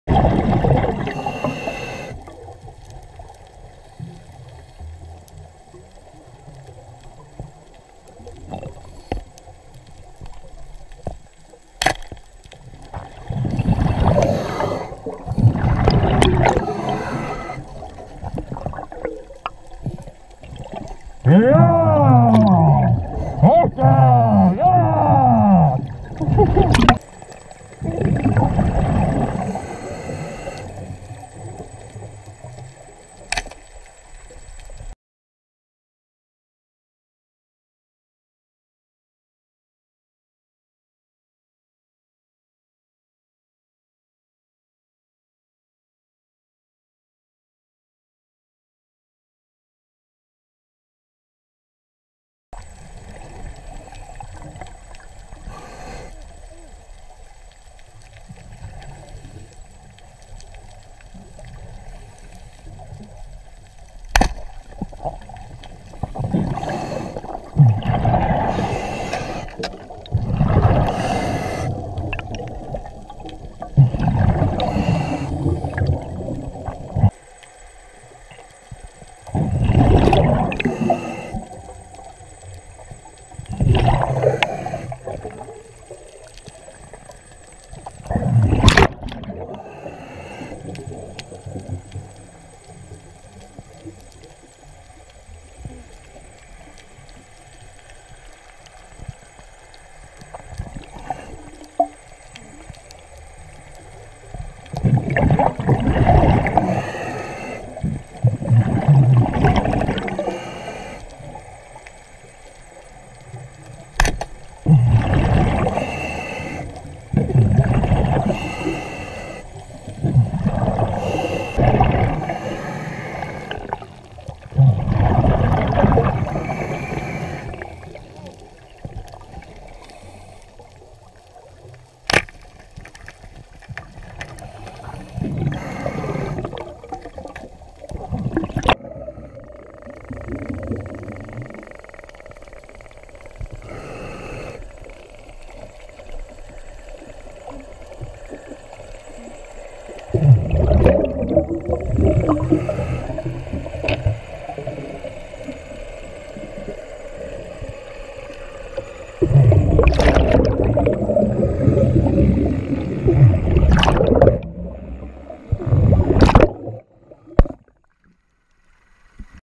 Four Hogfish Ledge
Four Ok Male Hogfish on a Ledge out of St. Pete. 27 foot Ameracat